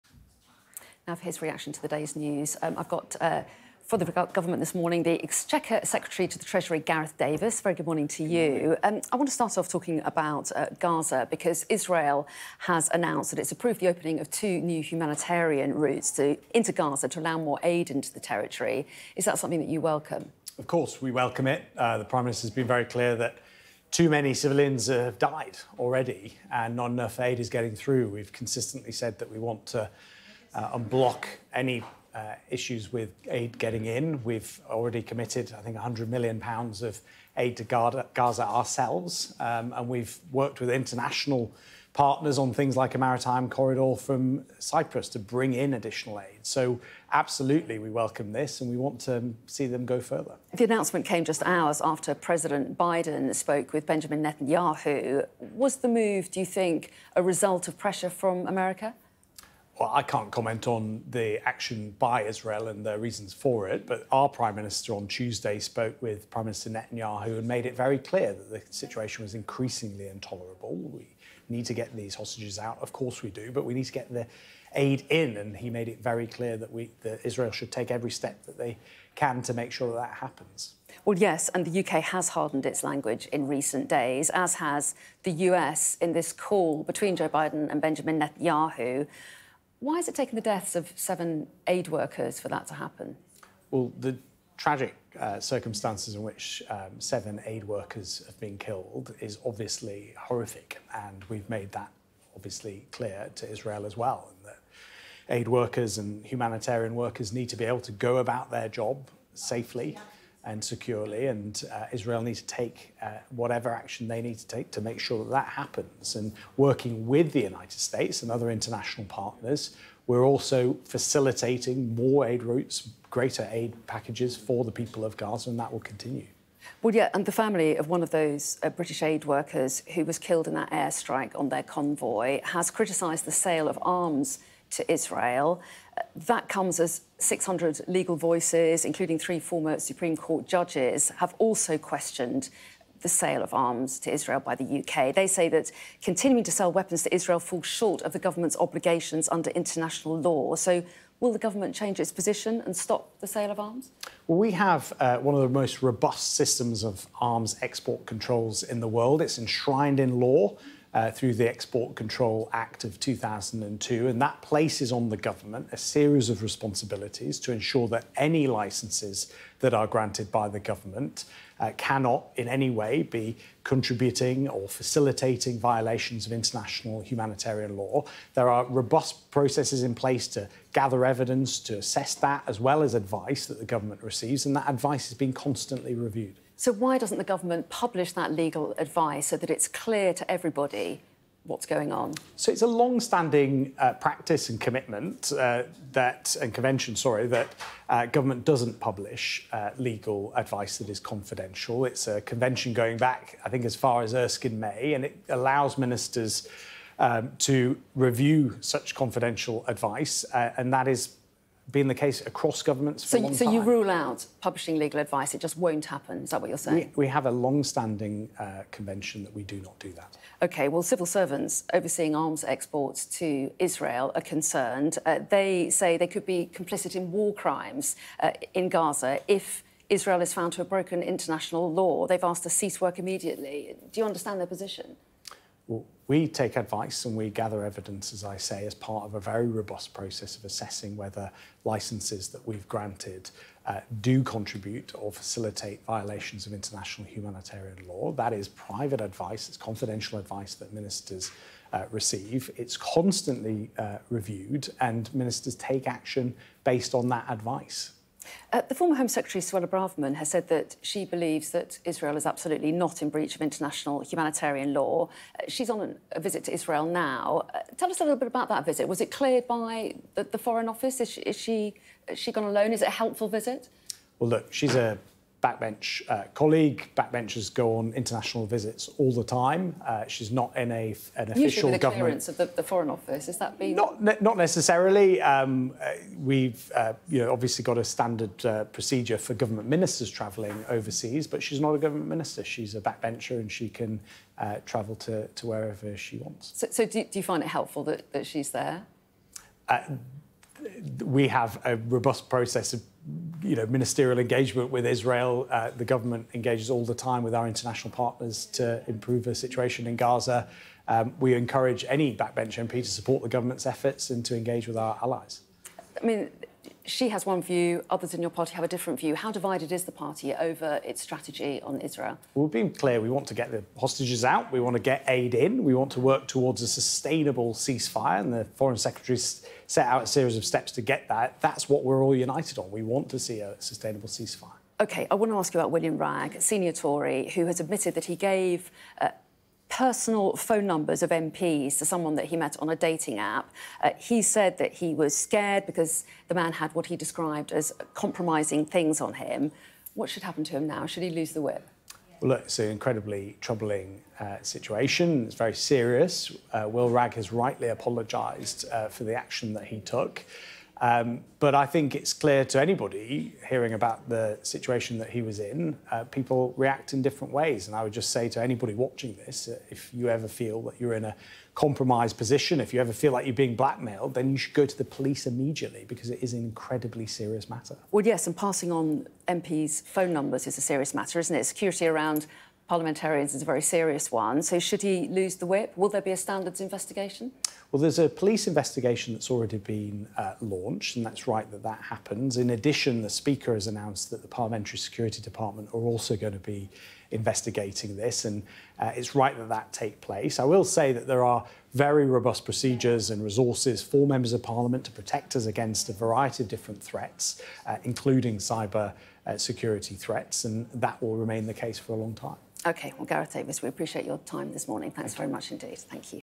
William Wragg dating app claims 'incredibly troubling situation', says Tory MP Gareth Davies
The Exchequer Secretary to the Treasury has defended a Conservative MP who, according to reports in The Times, gave out the personal phone numbers of colleagues to someone he met on a dating app.

Gareth Davies said William Wragg had "rightly apologised" but added "people react in different ways" in such situations.

Mr Wragg told The Times he had handed over the numbers because an unidentified individual had compromising material of him.

Mr Davies refused to say whether the senior Tory should lose the party Whip over the incident.